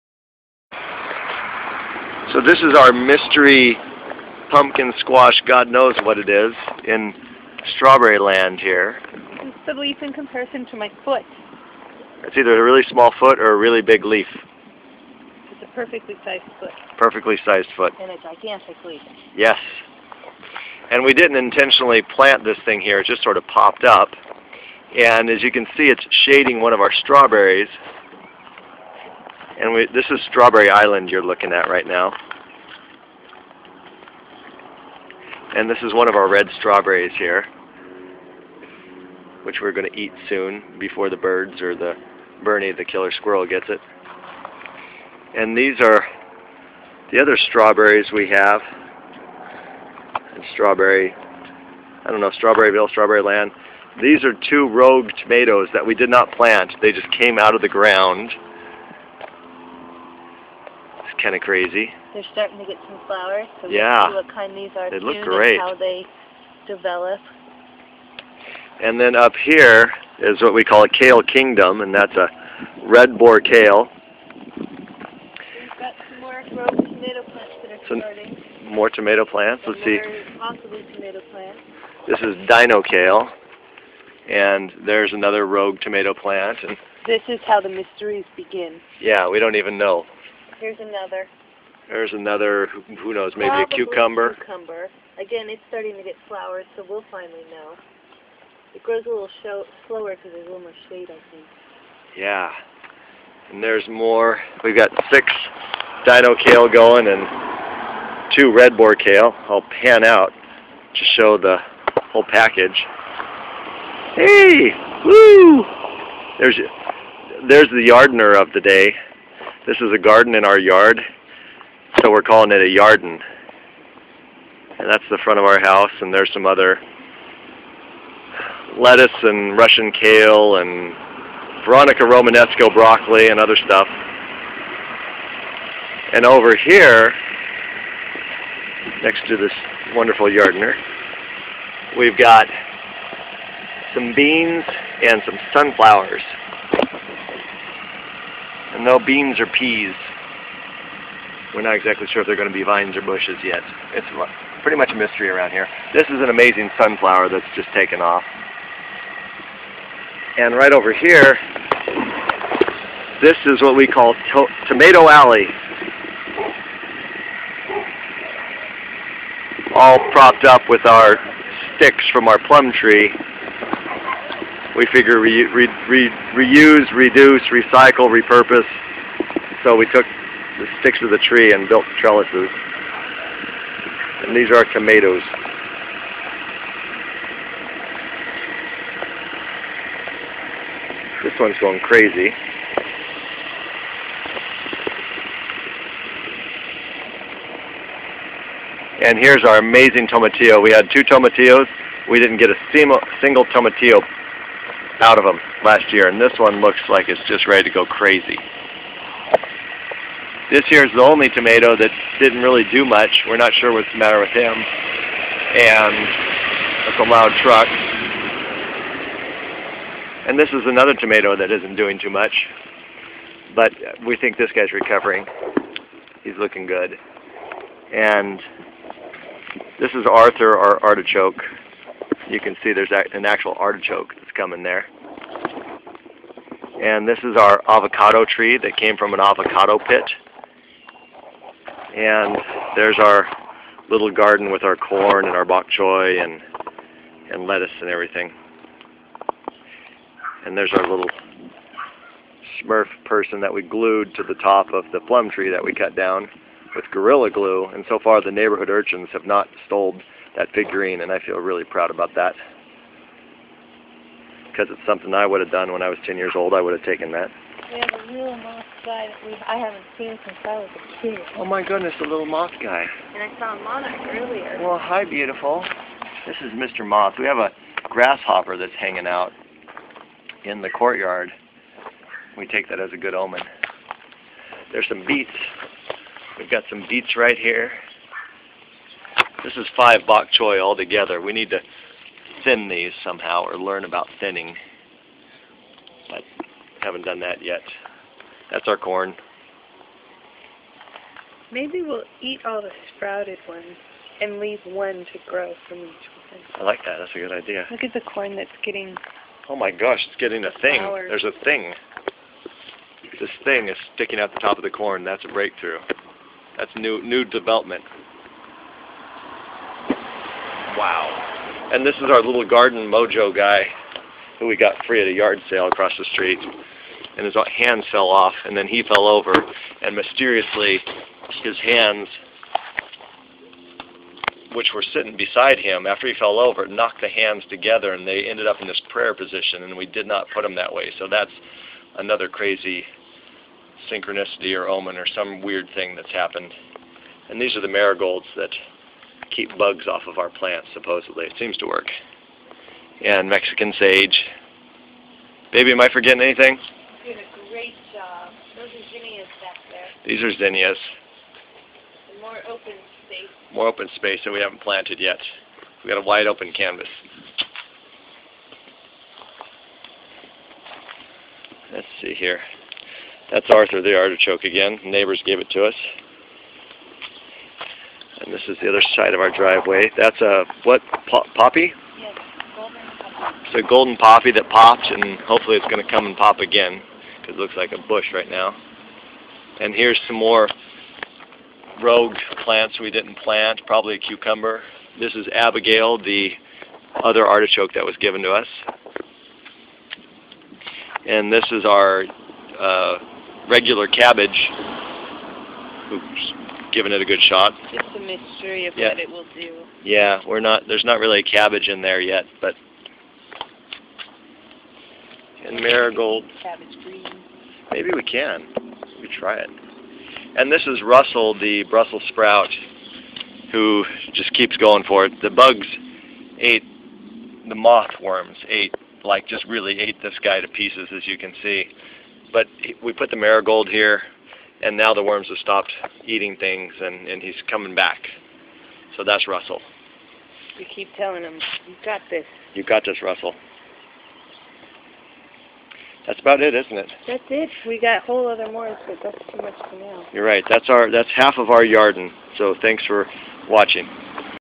Our Yarden As of May 2013
A little tour of the nature lab we've got in our front yard.